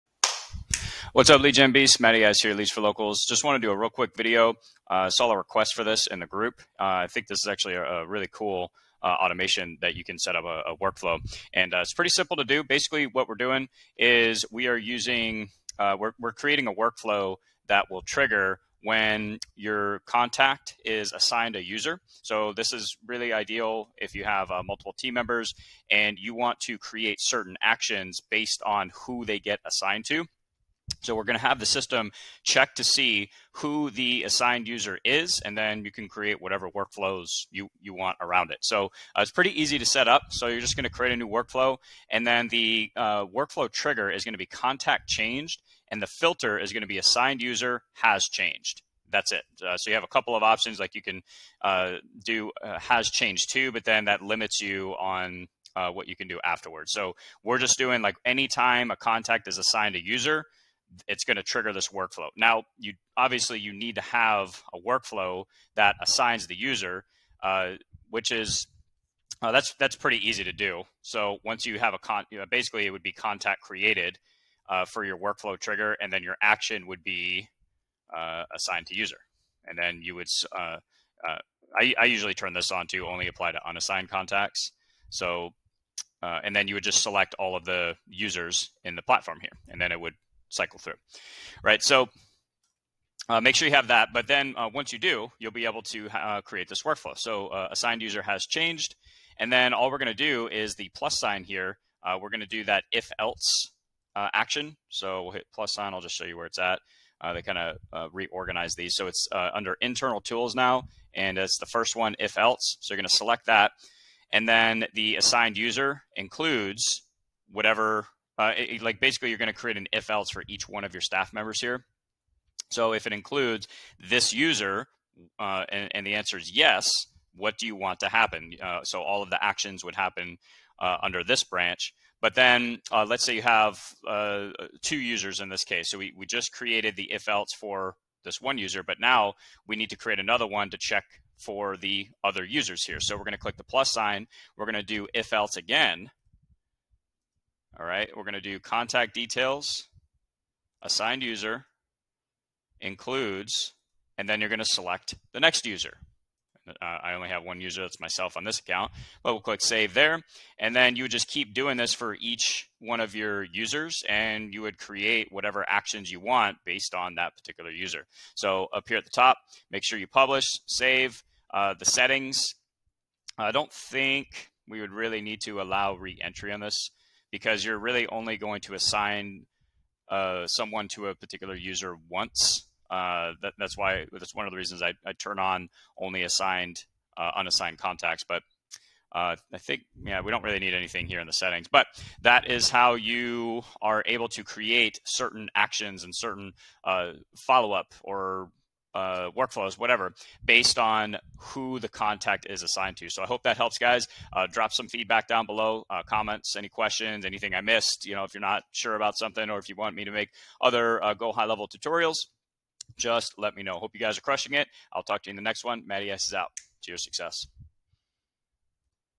GoHighLevel Workflow Tutorial✅ How to create workflows for specific Assigned Users
Time Stamp:
0:00 GoHighLevel Workflow for Assigned Users
In this GoHighLevel tutorial, I show you a simple workflow that will allow you to create specific actions based on who a contact gets assigned to.

This is great if you have multiple staff members in your GoHighLevel account and wants certain actions to take place when they are assigned a new user.